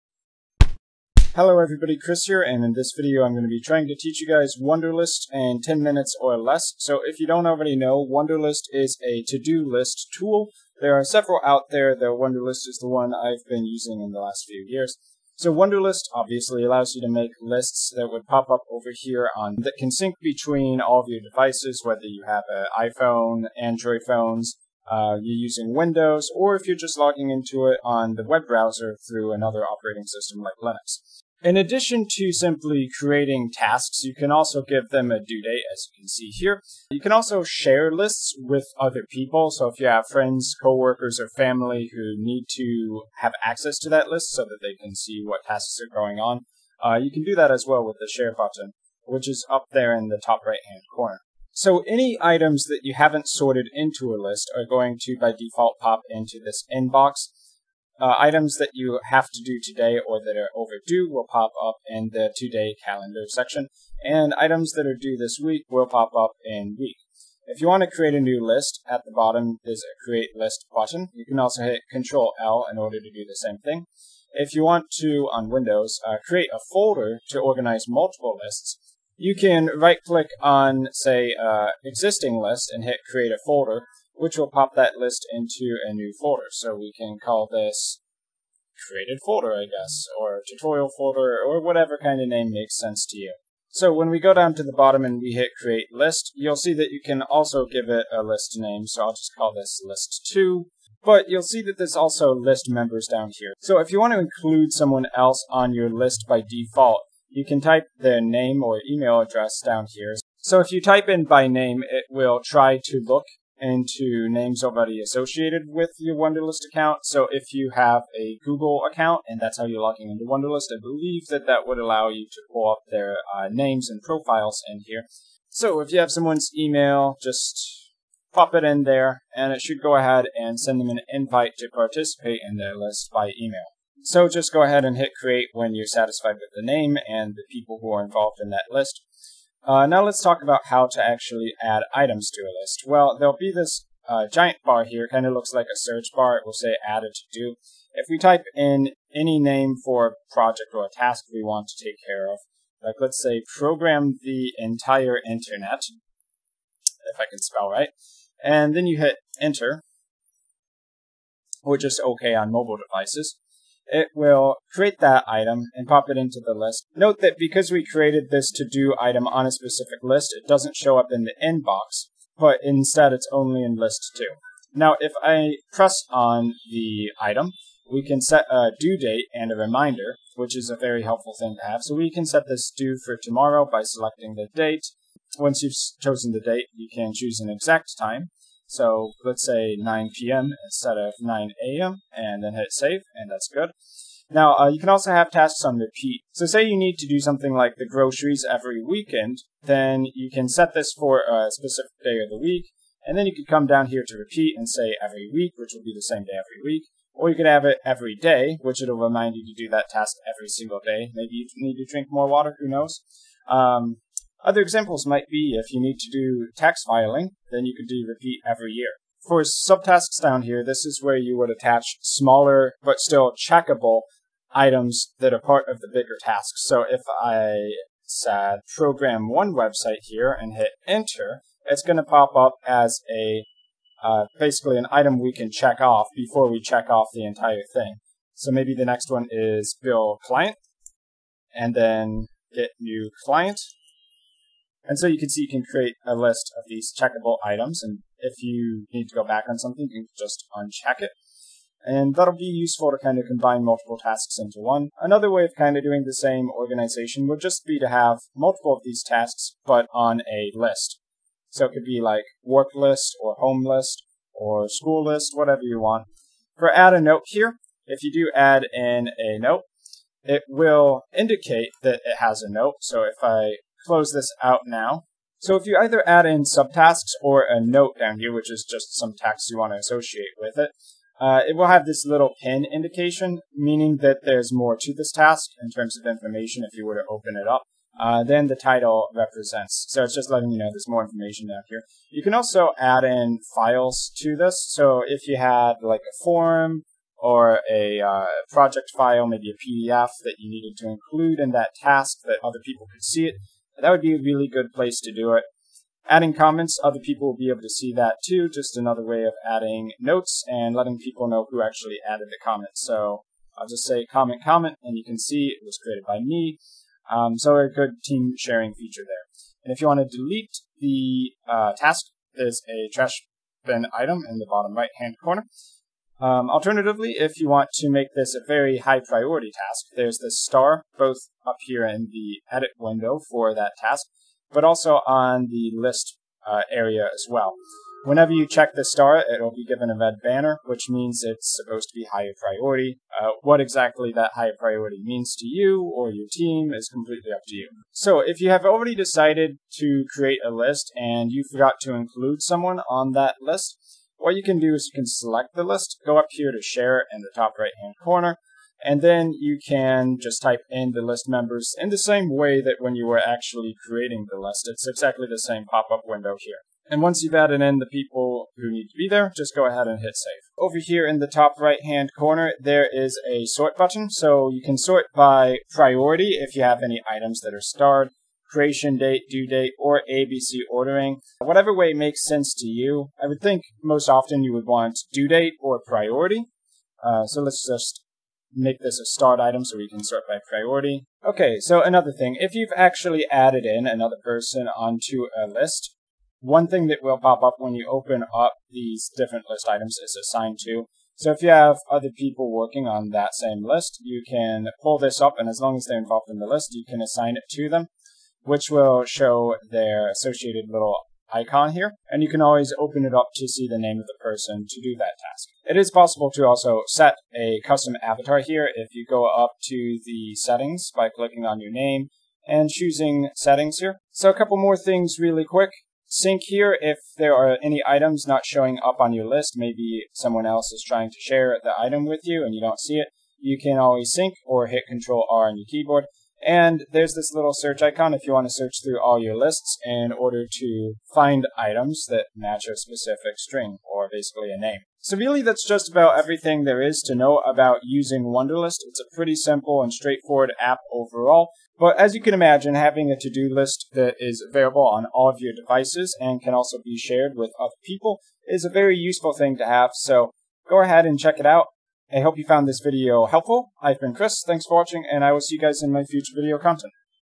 Learn Wunderlist in Under 10 Minutes - Best Free Todo List App 2018
Complete guide to learning Wunderlist, the best free todo list app. All important features covered up to it's 2018 versioning.

Bitcoin: 17Pn5PifFmRkHRAqCZQAom1ZjCiSf7qJjJ
Ethereum: 0x6f2781F382952c8caCDbE99C46F07c265ab59627
Steem: @christutorials